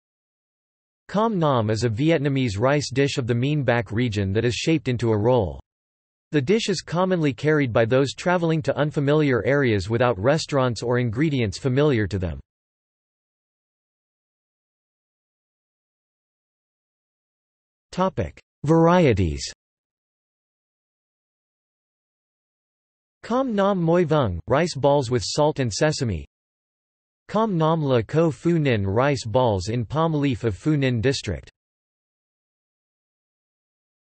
Cơm nắm | Wikipedia audio article
This is an audio version of the Wikipedia Article:
Cơm nắm

00:00:14 undefined

- Socrates

SUMMARY
Cơm nắm is a Vietnamese rice dish of the Miền Bắc region that is shaped into a roll. The dish is commonly carried by those traveling to unfamiliar areas without restaurants or ingredients familiar to them.